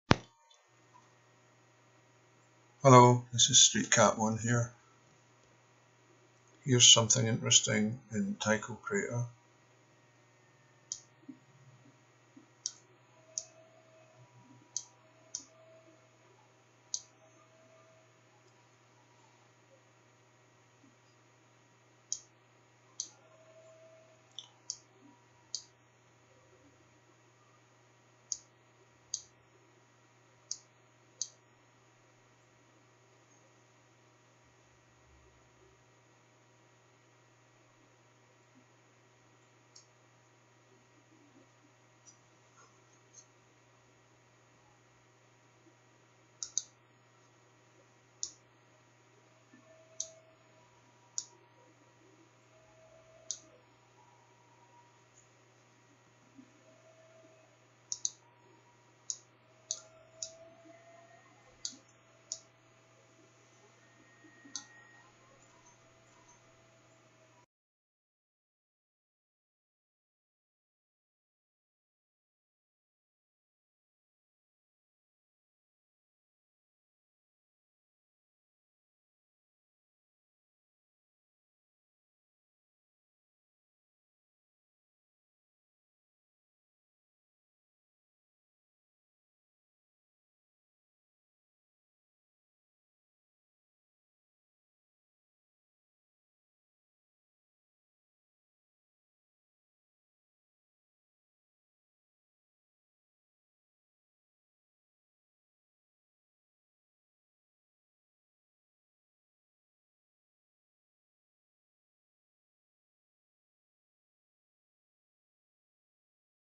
Tycho Crater on Earth's Moon has more Buildings
Be assured these are very real. This photo was taken from Lunar Orbit in 2016. Just how long they have been there is the real question.   Streetcap1.